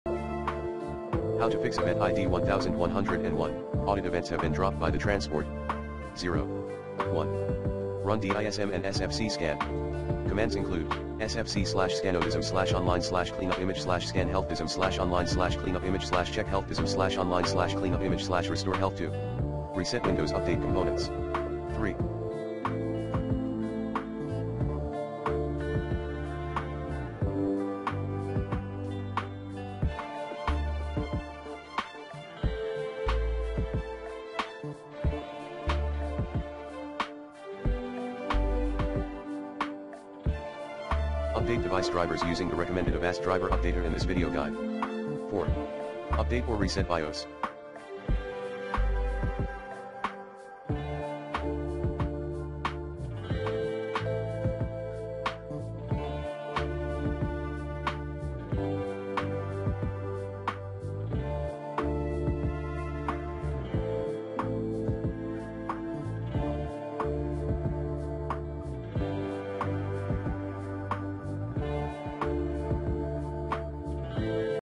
Fix Event ID 1101, Audit events have been dropped by the transport. 0 on Windows
Here's how to fix Event ID 1101, Audit events have been dropped by the transport. 0 on Windows. Run Avast Driver Updater@ to keep your device drivers up-to-date, easily & effectively.